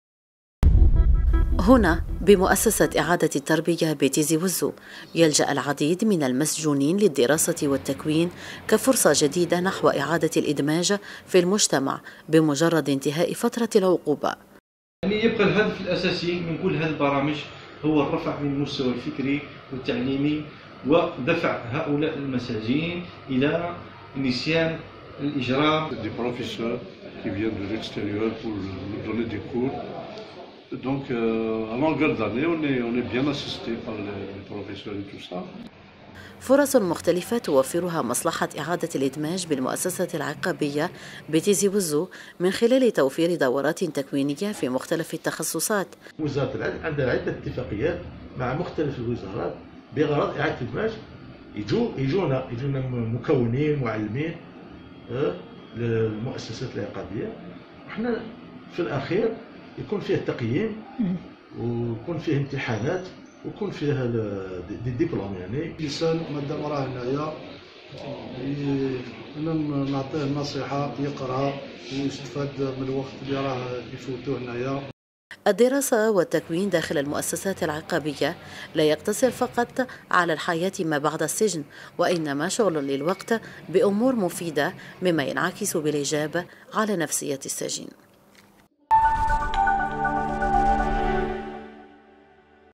تيزي وزو: مؤسسة إعادة التربية فرصة للدراسة و التكوين بهدف إعادة الادماج
تيزي وزو(وأج) - تمثل الدراسة والتكوين بالنسبة لنزلاء مؤسسة إعادة التربية والتأهيل لتيزي وزو, فرصة جديدة نحو إعادة الإدماج ورابطا يبقيهم في اتصال مستمر مع العالم الخارجي.